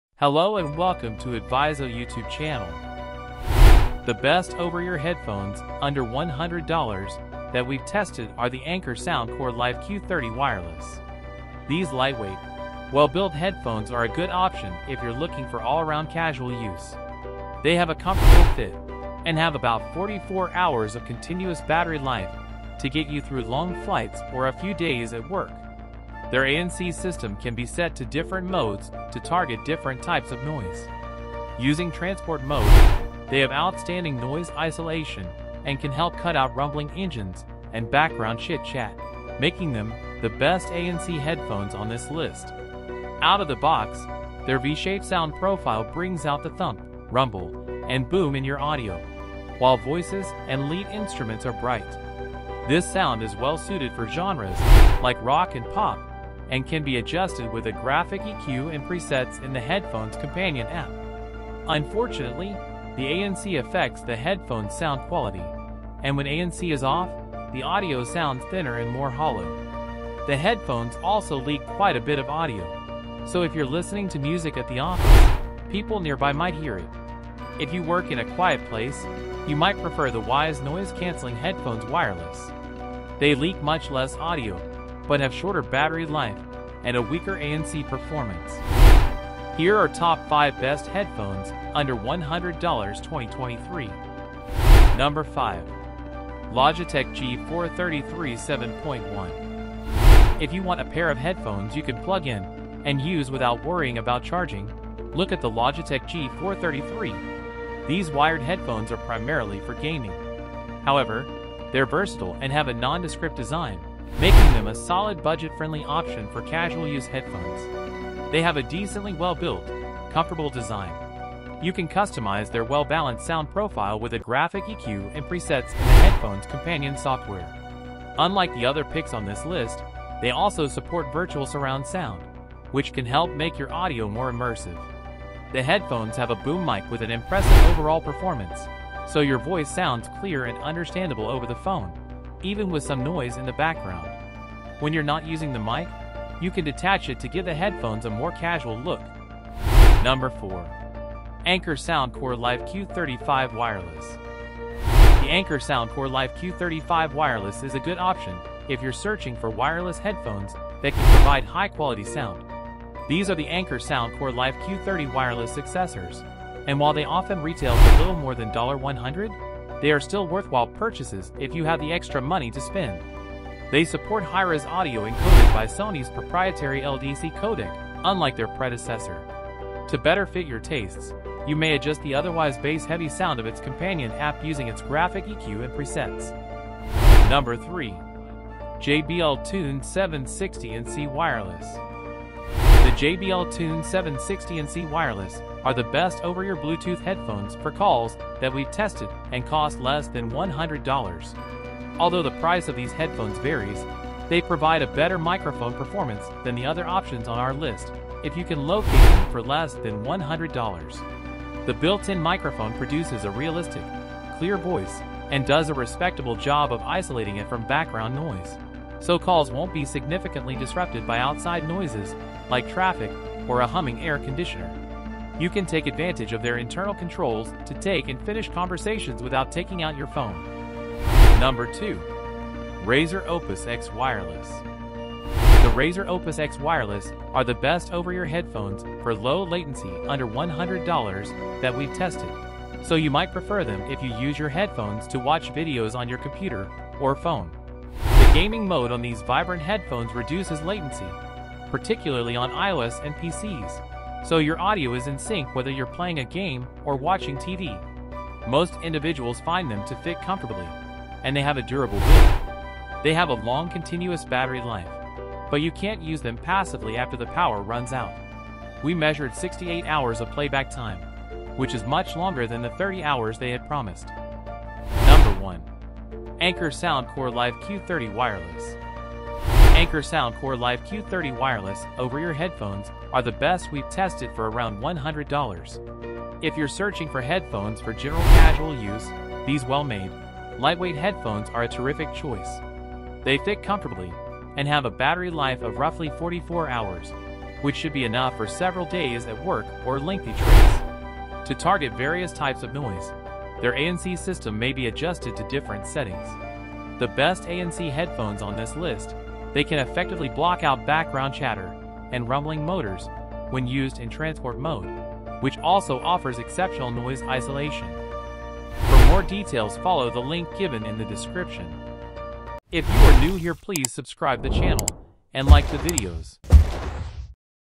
Top 5 Best Headsets Under $100 2024
You can buy Top 5 Best Headphones Under $100 2024  by following the links.

1. Anker Soundcore Life Q30 Wireless

2. Razer Opus X Wireless

3. JBL Tune 760NC Wireless

4. Anker Soundcore Life Q35 Wireless

5. Logitech G433